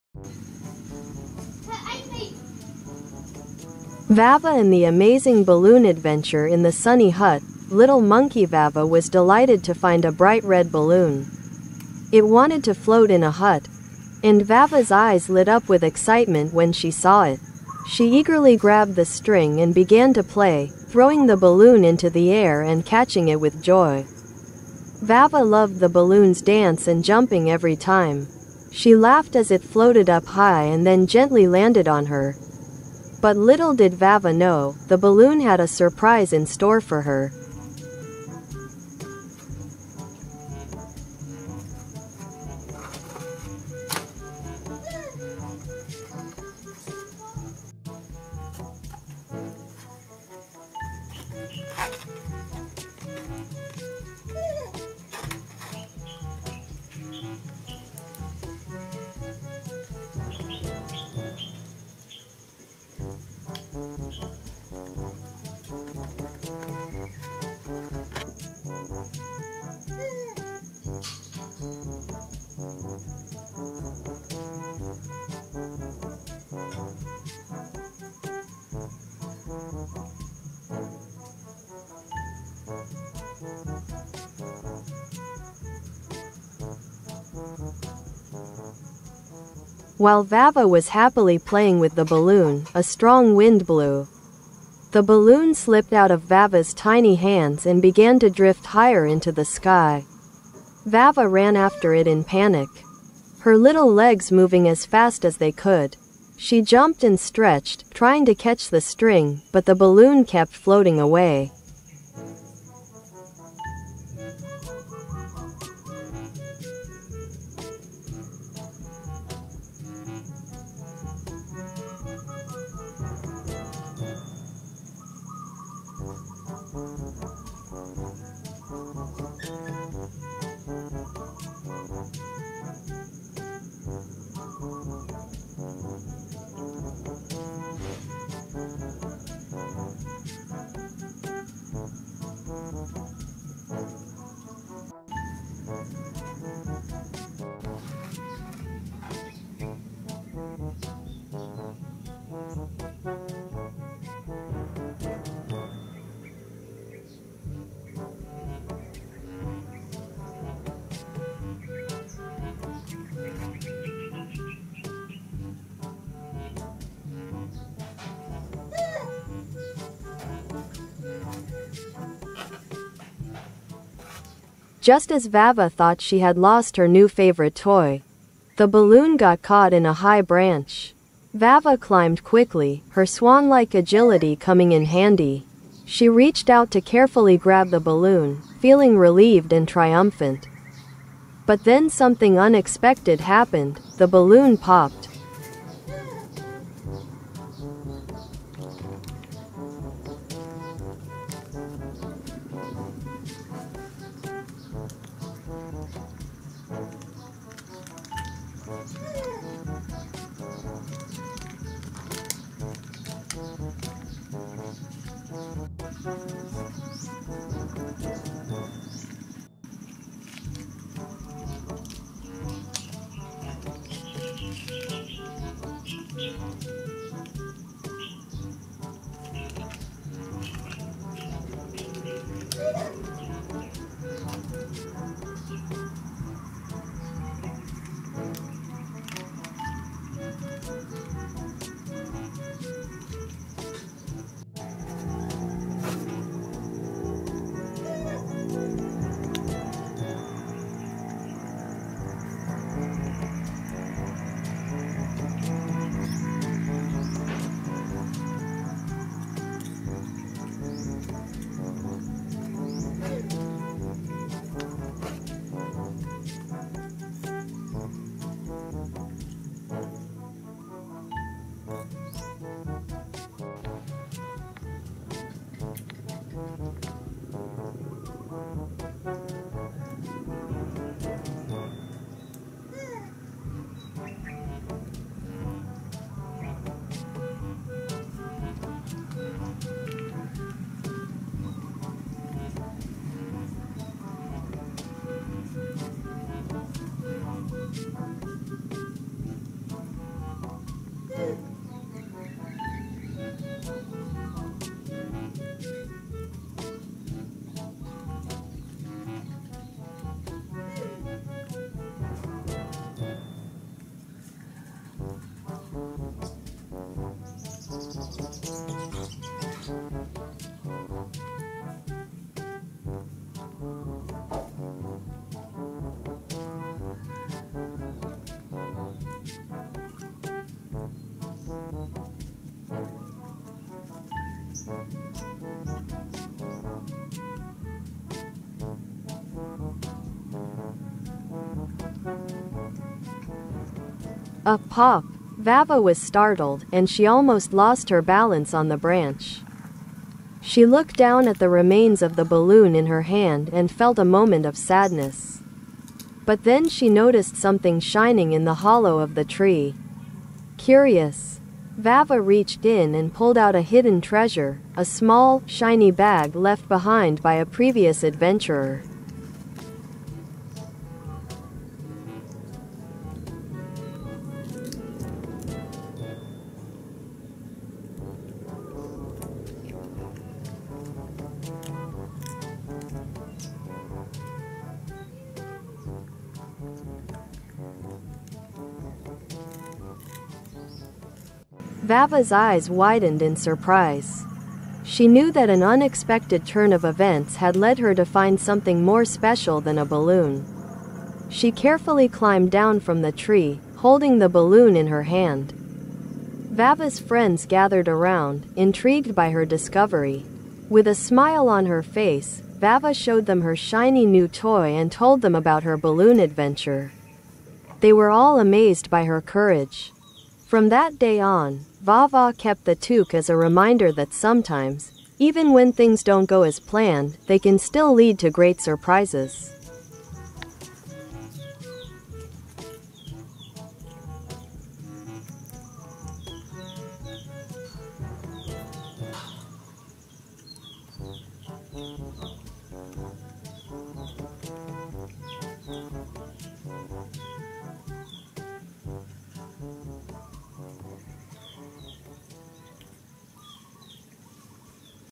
Cute monkey Va playing with balloon something the monkey didn't expect happened while playing it
VaVa and the Amazing Balloon Adventure

In the sunny hut, little monkey VaVa was delighted to find a bright red balloon. It wanted to float in a hut, and VaVa’s eyes lit up with excitement when she saw it. She eagerly grabbed the string and began to play, throwing the balloon into the air and catching it with joy.

VaVa loved the balloon’s dance and jumping every time. She laughed as it floated up high and then gently landed on her. But little did VaVa know, the balloon had a surprise in store for her.

While VaVa was happily playing with the balloon, a strong wind blew. The balloon slipped out of VaVa’s tiny hands and began to drift higher into the sky. VaVa ran after it in panic, her little legs moving as fast as they could. She jumped and stretched, trying to catch the string, but the balloon kept floating away.

Just as VaVa thought she had lost her new favorite toy, the balloon got caught in a high branch. VaVa climbed quickly, her swanlike agility coming in handy. She reached out to carefully grab the balloon, feeling relieved and triumphant. But then something unexpected happened — the balloon popped!

A “POP!” VaVa was startled, and she almost lost her balance on the branch. She looked down at the remains of the balloon in her hand and felt a moment of sadness. But then she noticed something shining in the hollow of the tree. Curious, VaVa reached in and pulled out a hidden treasure, a small, shiny bag left behind by a previous adventurer.

VaVa’s eyes widened in surprise. She knew that an unexpected turn of events had led her to find something more special than a balloon. She carefully climbed down from the tree, holding the balloon in her hand. VaVa’s friends gathered around, intrigued by her discovery.

With a smile on her face, VaVa showed them her shiny new toy and told them about her balloon adventure. They were all amazed by her courage. From that day on, Va Va kept the toque as a reminder that sometimes, even when things don't go as planned, they can still lead to great surprises.